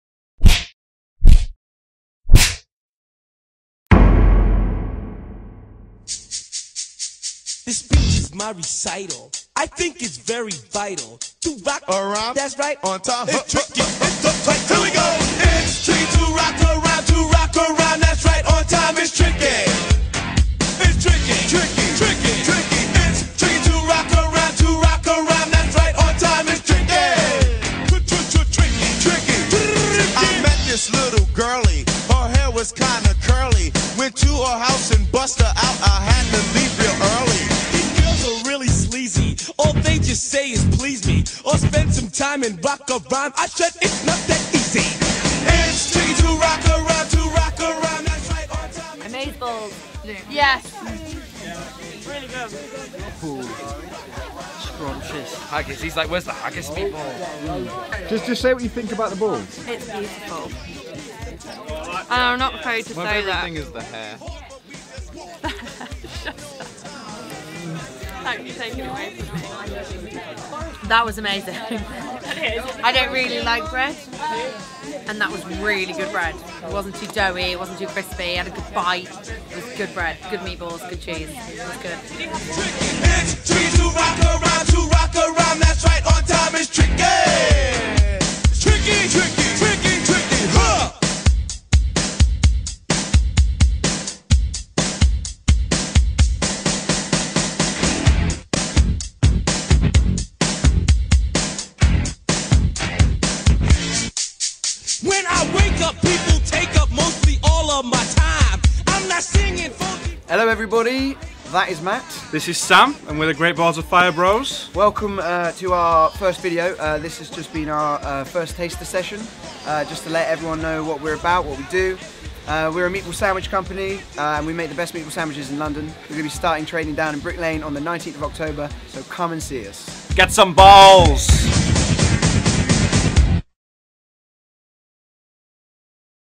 Great Balls of Fire Bros.
The Great Balls of Fire Bros are a new meatball company based in East London.

Find them every Saturday at the boiler house food hall on Brick Lane